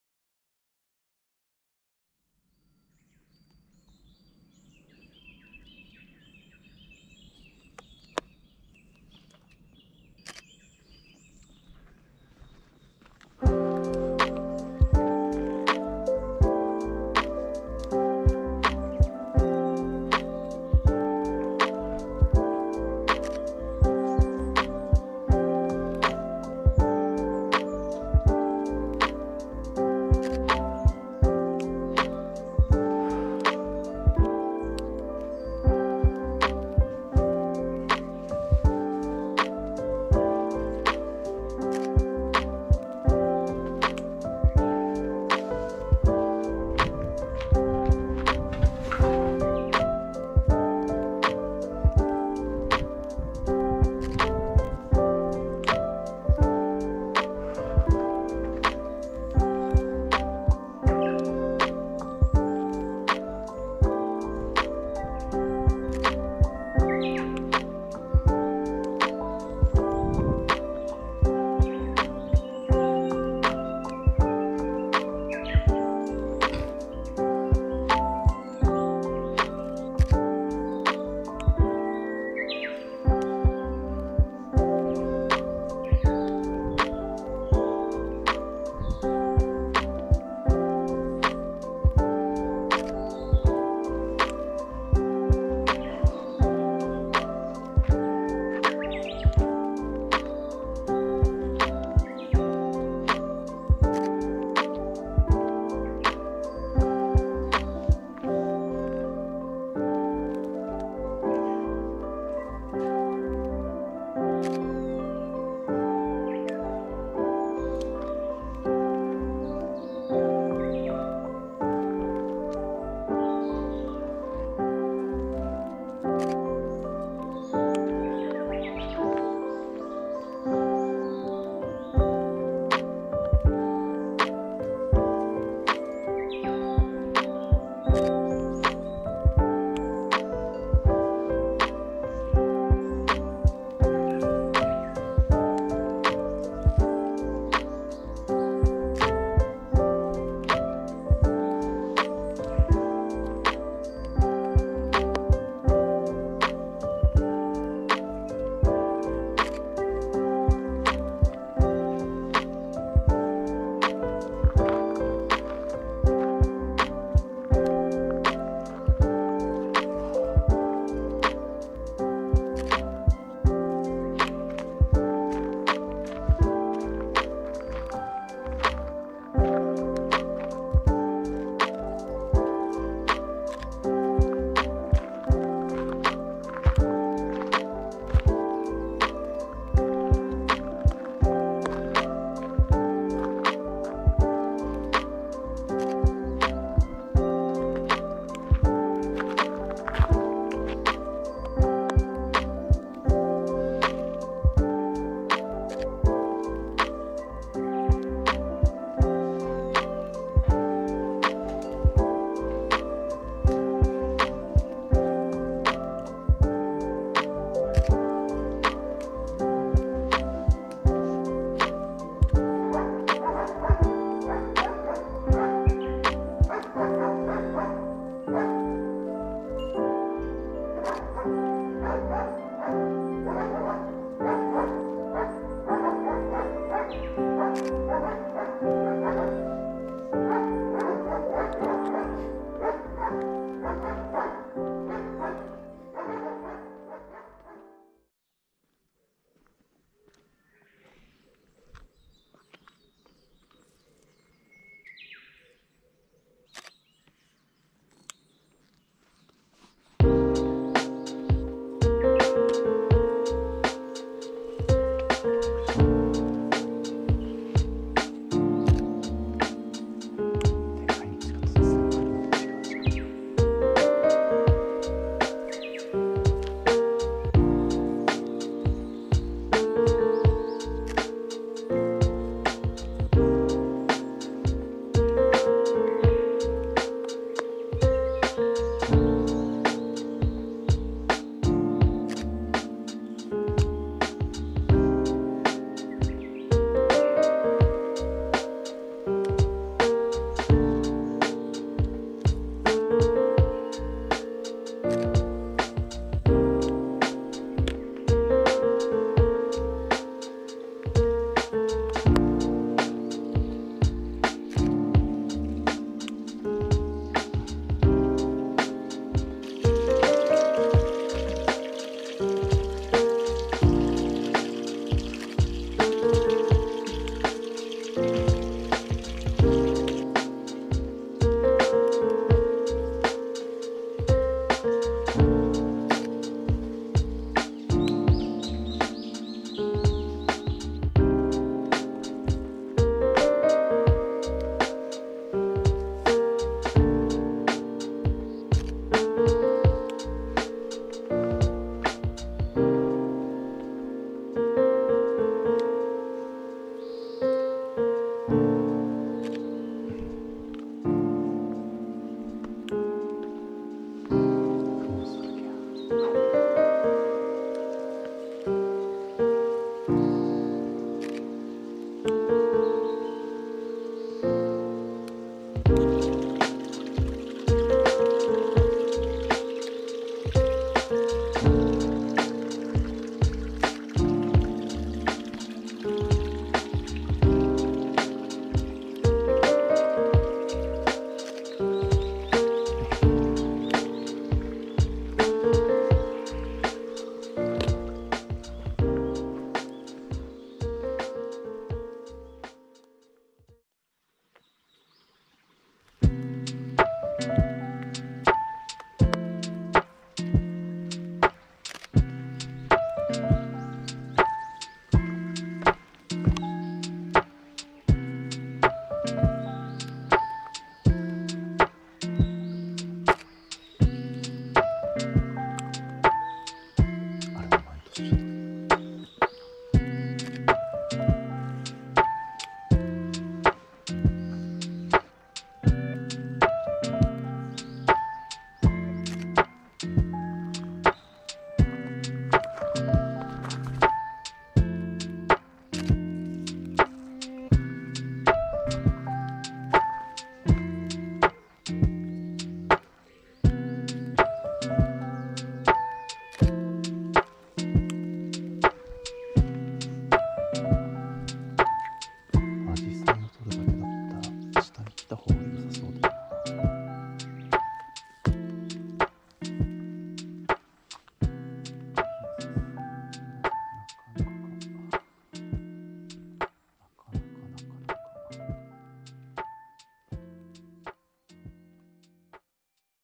POV Photography in Japan | Tochigi 栃木県栃木市 | June 2024
POV  Photography

栃木県栃木市
Tochigi City, Tochigi Prefecture

何度目になるか大平山。
今年は下から一気に上がってみたけどだいぶきつかった。
日が昇ったばかりの早朝なのに結構人がいる。
近所の人で、きっと毎日登っているんだろうな。

【Camera】
FUJIFILM X-Pro3
Voigtlander

【BGM】
zukisuzuki BGM